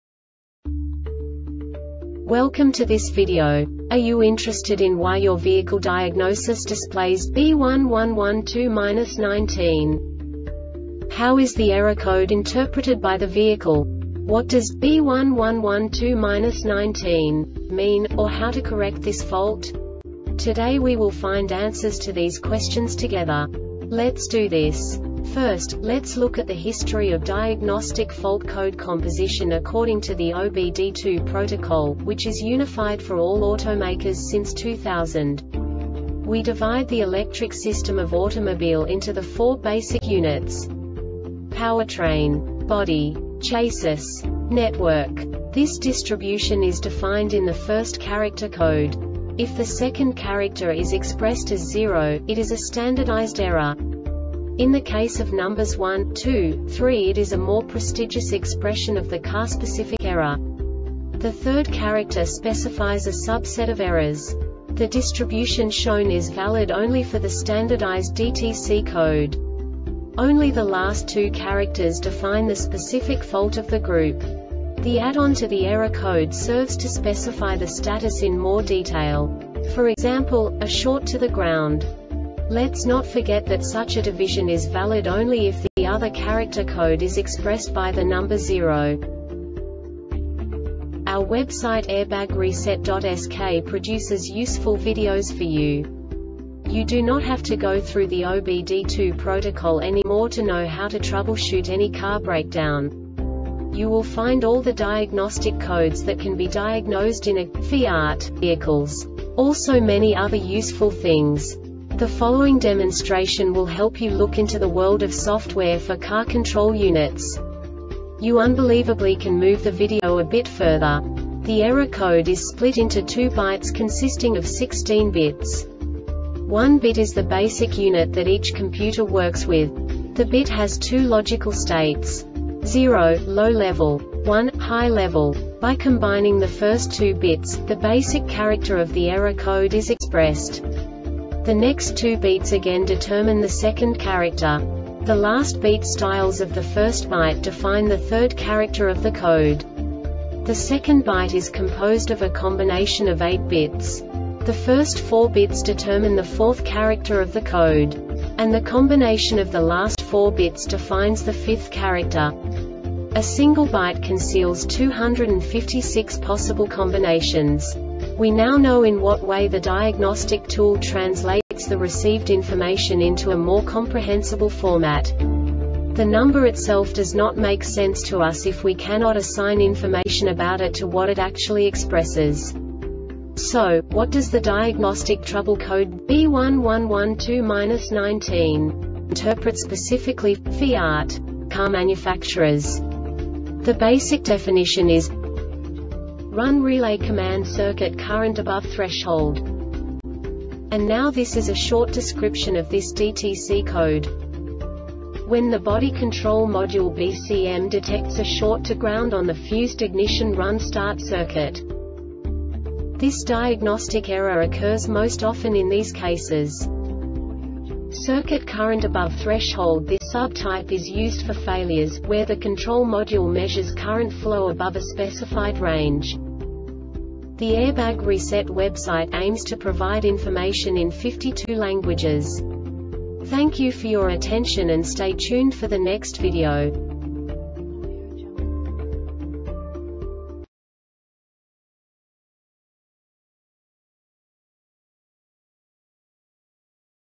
DTC Fiat B1112-19 Short Explanation
Contents:
0:21 Basic DTC analysis according to OBD2 protocol standard.
1:48 Insight into programming
2:58 A diagnostically understandable description of the Diagnostic Trouble Code
3:05 Basic error definition

Make:
FIAT
Code:
B1112-19
Definition:
Run Relay Command-Circuit Current Above Threshold
Description:
When the Body Control Module (BCM) detects a short to ground on the Fused Ignition Run or Start circuit.